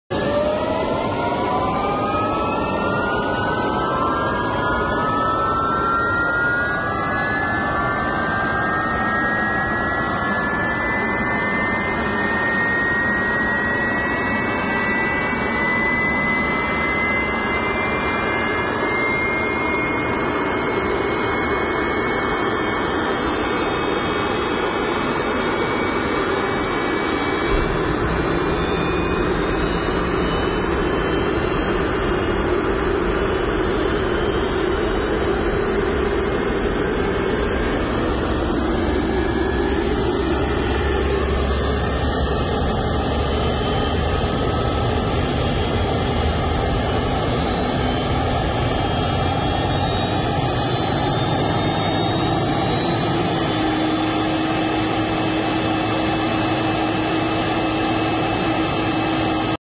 START UP 2 CLOSE Aircraft Jet Turbine Engine aviation video for you
👁 ❤️ ✈️  🎣  This Small and very Powerful  Engine is installed on the Hawker Jet Aircraft  model Cargo Airplane.  It is used for Humanitarian relief efforts for Ukraine and other countries at war times.
 Model  CF6-80 Thrust Rating at 62,000 MAX LBS

Hope you enjoyed short video.

Valuetainment